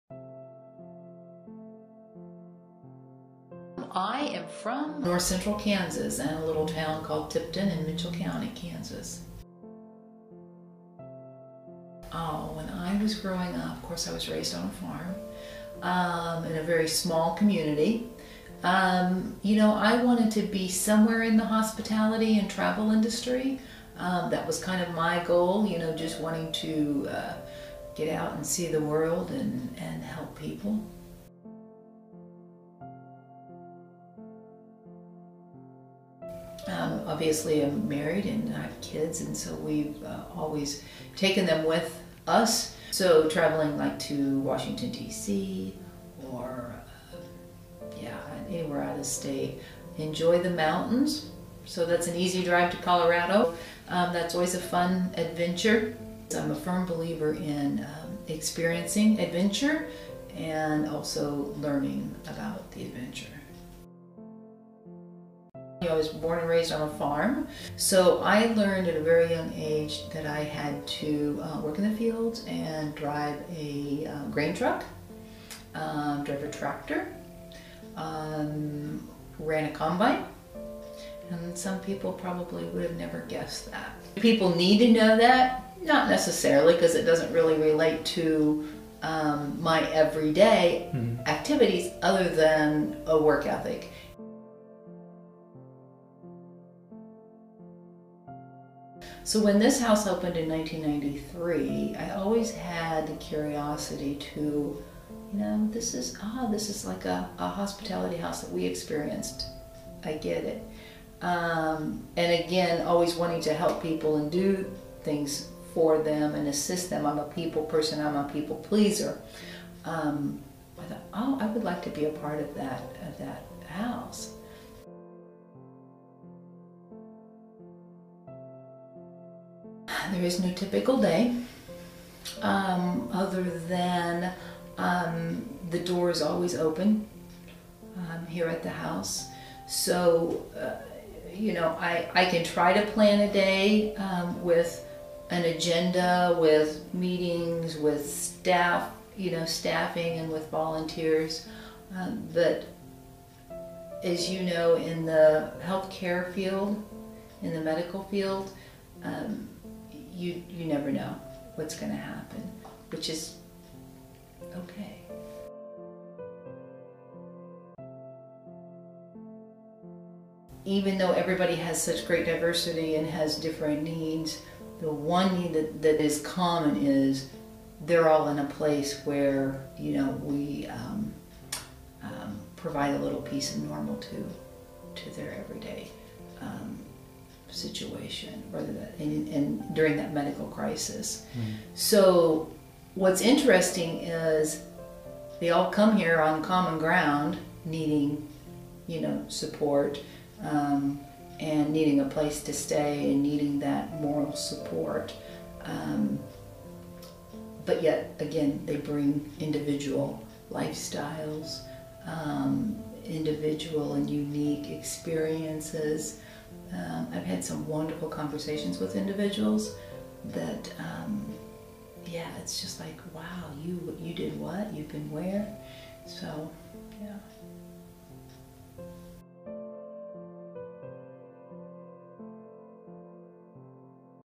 The Story of Lisa Hoffman
Mini-Documentary created for Digital Literacy
Special thanks to Lisa Hoffman and Helen Chipperfield for participating in this Documentary

Song by ShadyDave from freesound.org which I looped and cut to fit the video length
This video was not endorsed in any way.